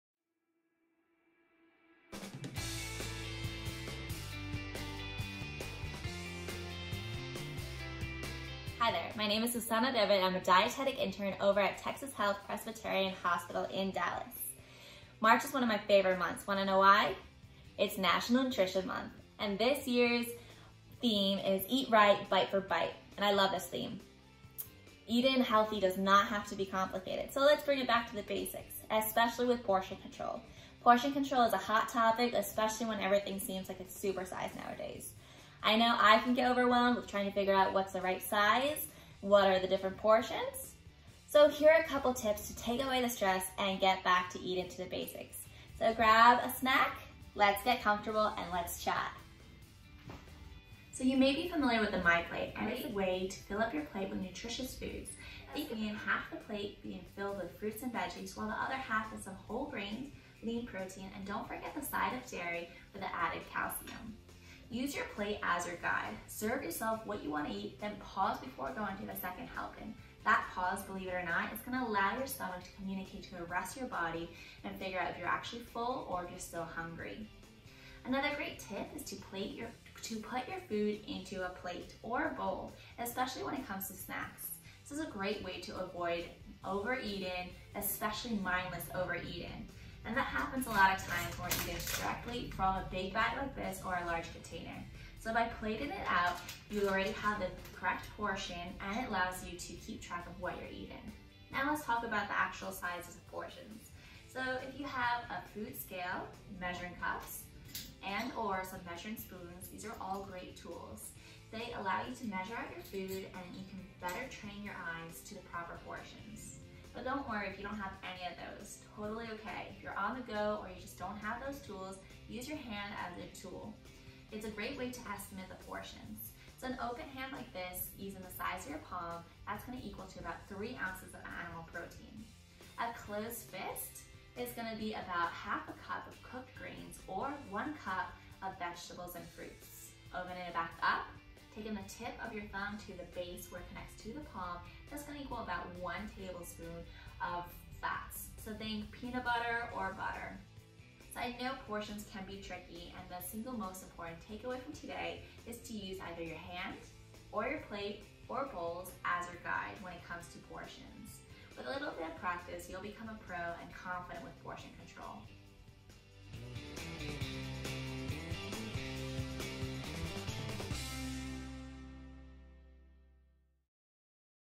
Back to the Basics: Eat Right, Bite by Bite
March is National Nutrition Month and this year’s theme is “Eat Right, Bite by Bite.” Susana Debbe, a dietetic intern at Texas Health Presbyterian Hospital in Dallas, says this theme can help us take eating back to the basics. Using the USDA’s MyPlate, she shows how easy it is to choose foods and portions for your plate to create nutritious and delicious meals.